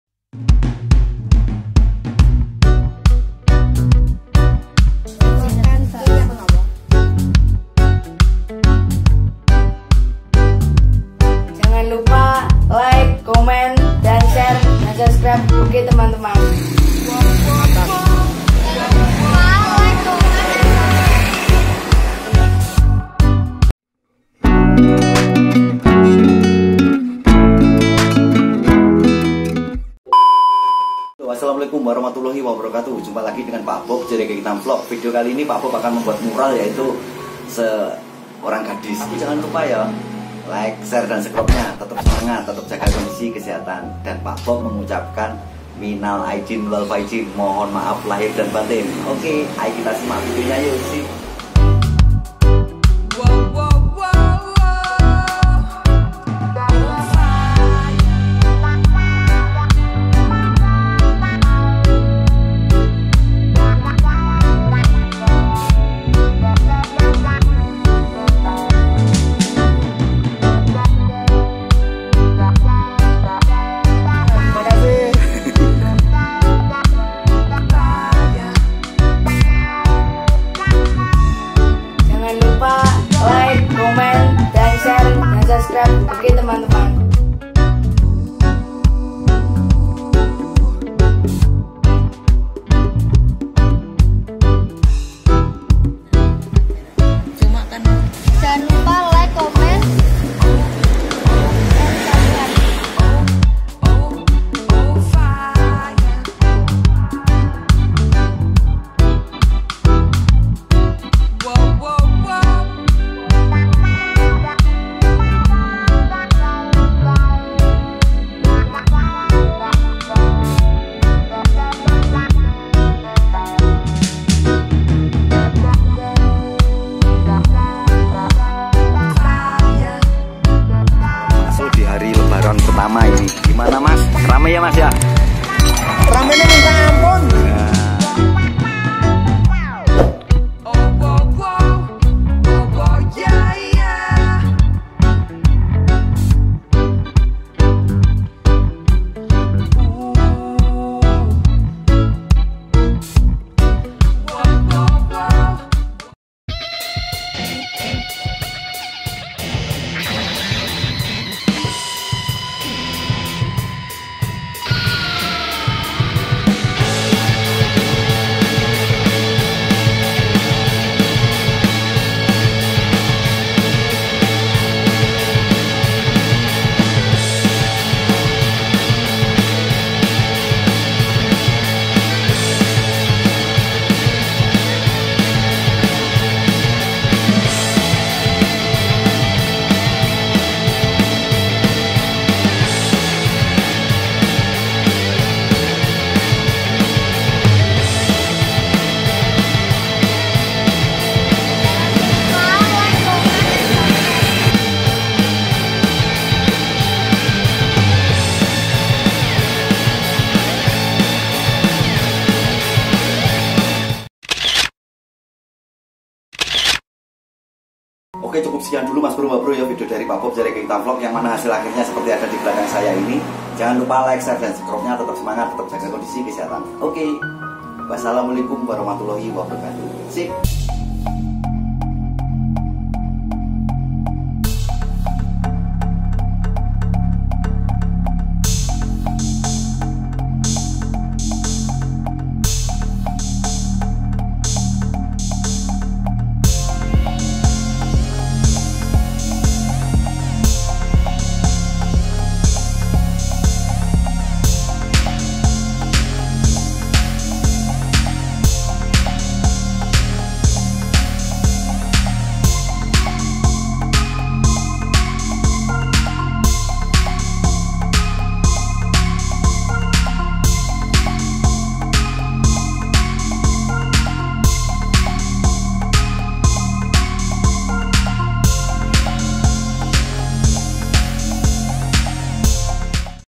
Simple Technique to Make 3D Murals Character Illustration | Mural Art
simple techniques for making a 3d mural character illustration || video mural art this time I give tricks and tips for making a 3d mural illustration of a character on the wall and simple techniques for making a 3d mural character illustration || mural art is very easy if you look carefully and for friends I can practice this and I use wall paint which is basically skin brown with a combination of black and the processing time is 3 hours 30 minutes. I hope this mural can help to make a simple mural, enjoy watching, thanks

# $ technique createmural3d
# wayamembuatmural3d

use the tubebuddy application so it's easy to find keywords or video tags. Blog Keyword Tool